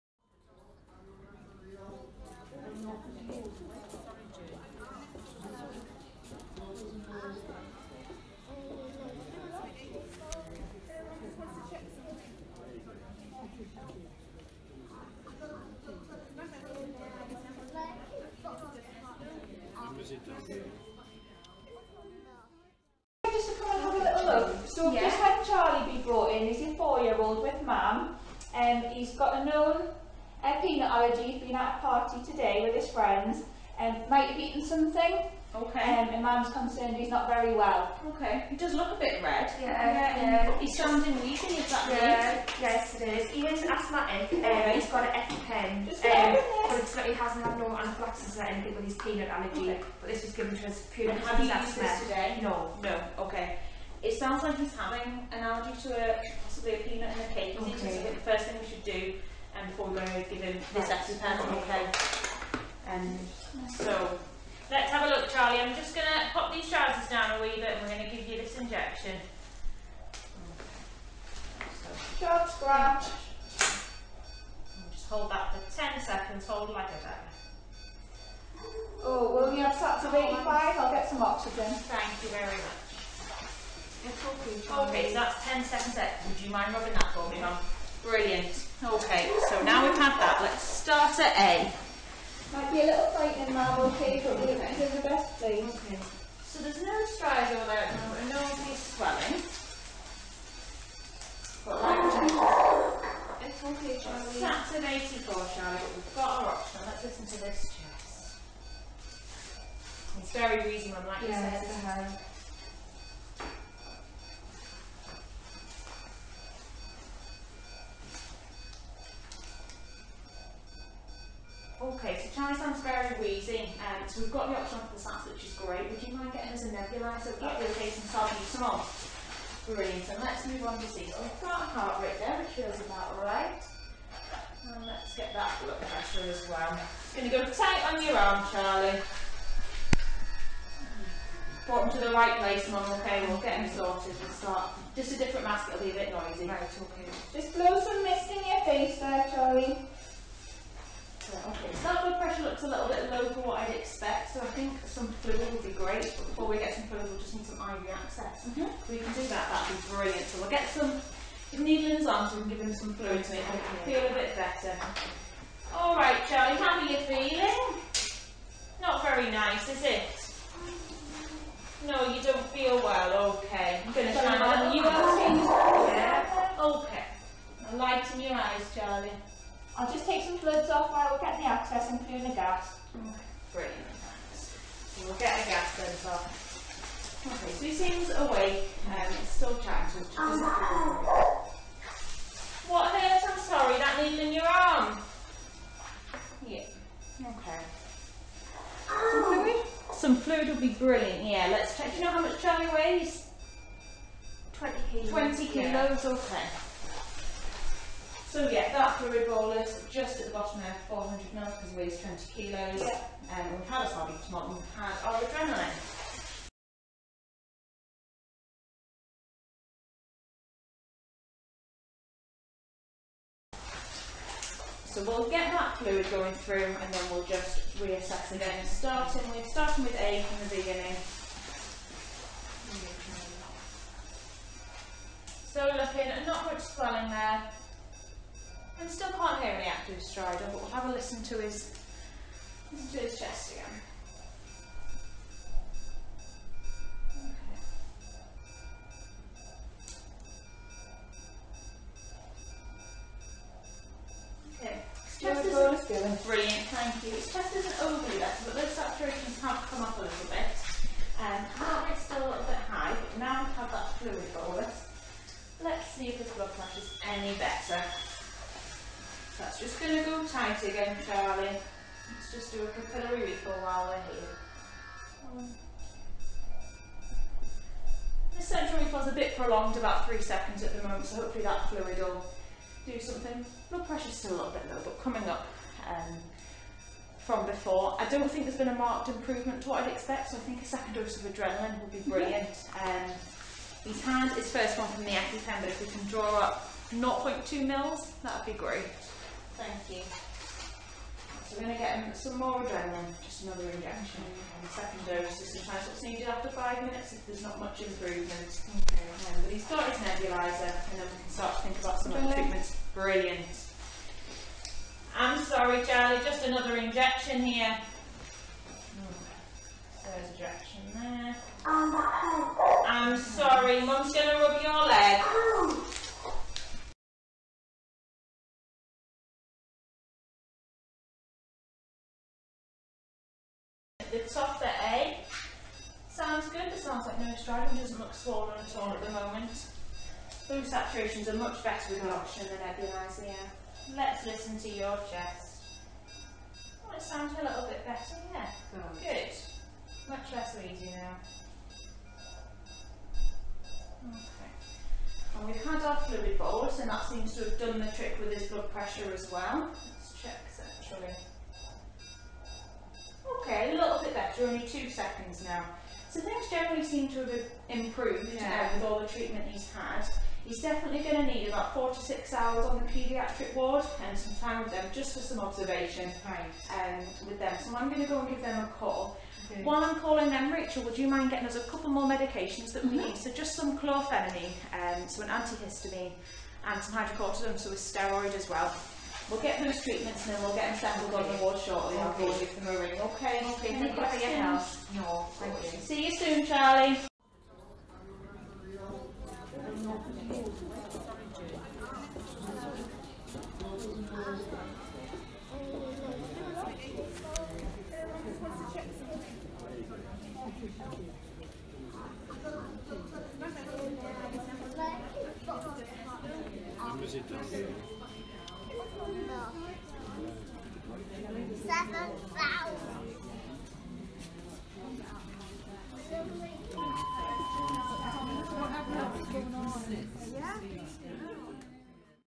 Anaphylaxis SIM Scenario 3 - Emergency Department
This simulation scenario was created as part of an educational package for health professionals to improve knowledge and understanding of how to manage anaphylaxis in children. This scenario is set in the resuscitation area of a hospital's Emergency Department. This child has presented with breathing difficulties after eating food at a party.